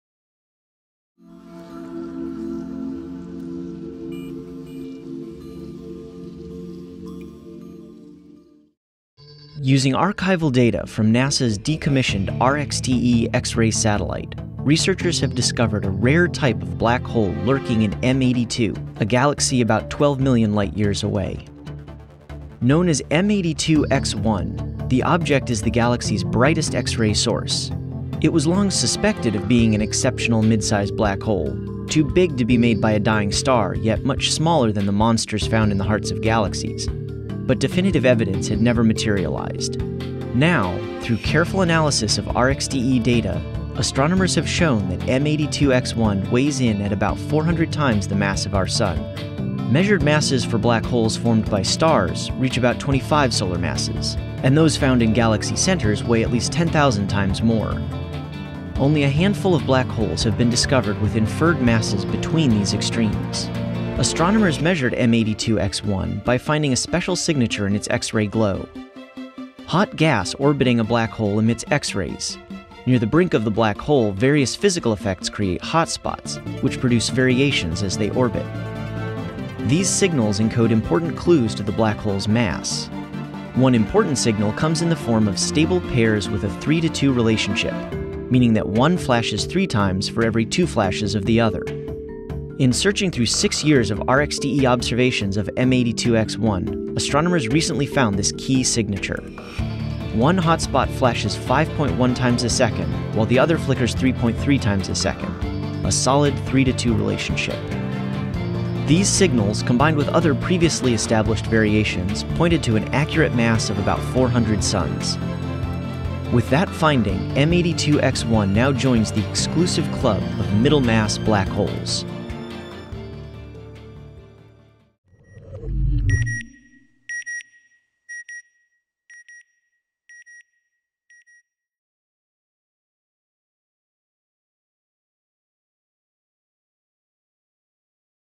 ألمع مصدر للأشعة السينية في المجرة M82 الثقب الأسود X-1
النظام النجمي (M82 X-1) هو ألمع مصدر للأشعة السينية في المجرة (M82)
وهي مجرة ​​تقع على بعد حوالي 12 مليون سنة ضوئية في كوكبة الدب الأكبر.
علماء الفلك يعتقدون أن النظام (M82 X-1) يحتوي ثقب أسود يزن حوالي 400 كتلة شمسية
ويعتبر من الثقوب السوداء متوسطة الحجم أو متوسطة الكتلة.
النظام النجمي (M82 X-1) نظام يتكون من ثقب أسود ونجم ضخم يولد تيارات من الغاز
باتجاه الثقب الأسود وتتراكم حول قرص الثقب الأسود.
الاحتكاك داخل القرص يسخن الغاز إلى ملايين الدرجات،
حيث تصبح ساخنة بما يكفي لتنبعث منها الأشعة السينية.
والتغيرات الدورية في شدة هذه الأشعة السينية تعكس العمليات التي تحدث داخل القرص.